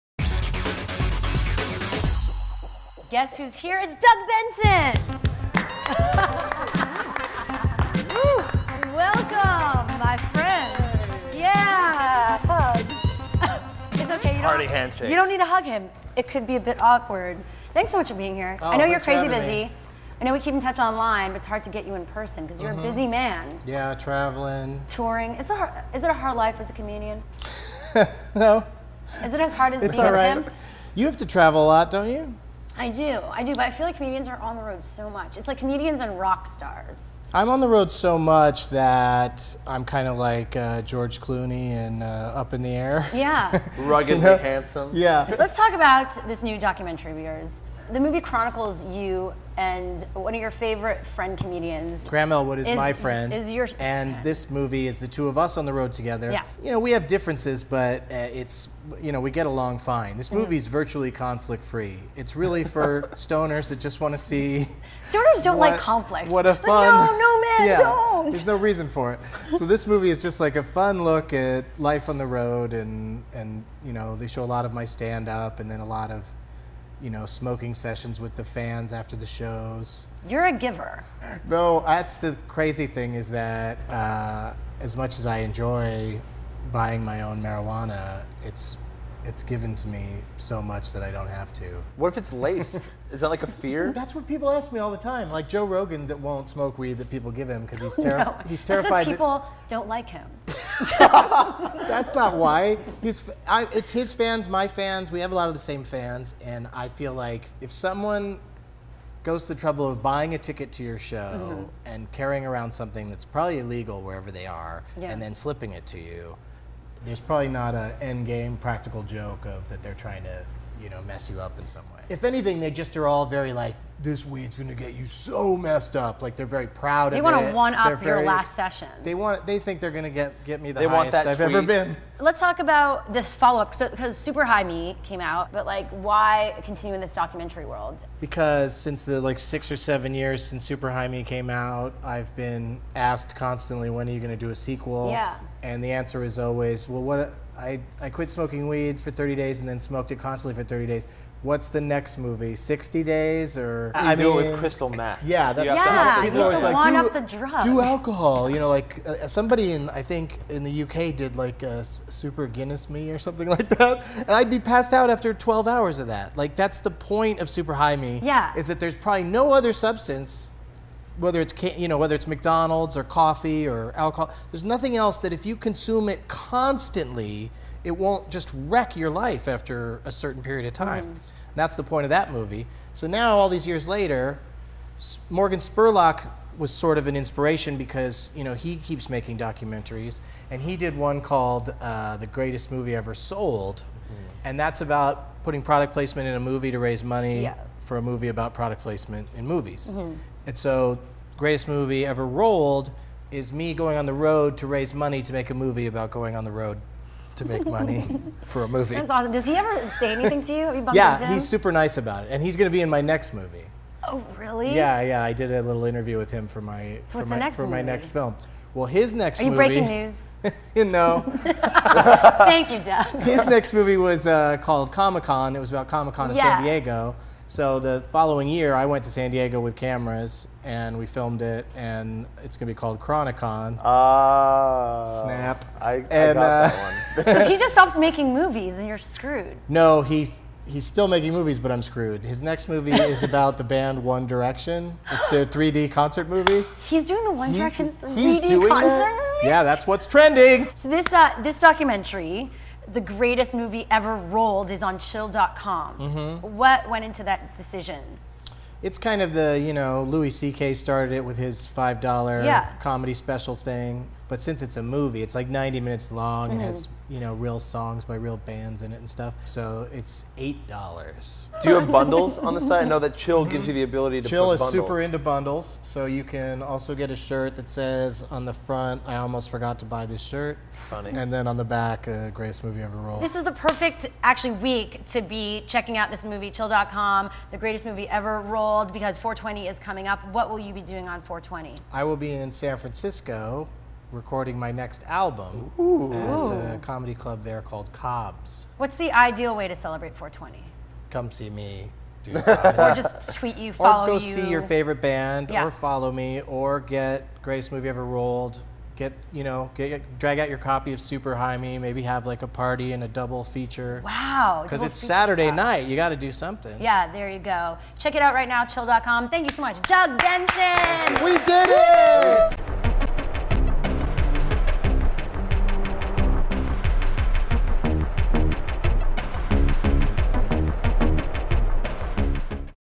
Doug Benson's 420 Plans PLUS "The Greatest Movie Ever Rolled" and "Chronic-Con" Preview!
Comedian Doug Benson on 420 movie marathons, his similarity to George Clooney and following up his smash hit documentary "Super High Me."

Hey, everybody! Doug Benson's documentary "Super High Me" followed the comic as he gave up weed smoking entirely for a month (the horror!) and then smoked constantly for another month. Ever since, he's been scanning about for his next great movie idea. Now, Benson's releasing "The Greatest Movie Ever Rolled," exclusively on Chill, and yet again he's been inspired by fellow documentarian Morgan Spurlock!  This time, Benson and partner-in-crime Graham Elwood are touring the nation, doing comedy and raising money to make a documentary... about touring the nation doing comedy. Wait... hang on... yeah, that's right.

Benson sat down with Shira Lazar to talk about making the movie, how touring constantly isn't really that big a deal and his follow-up to "Greatest Movie Ever Rolled," a stoned journey to San Diego Comic-Con titled... wait for it... "Chronic-Con!"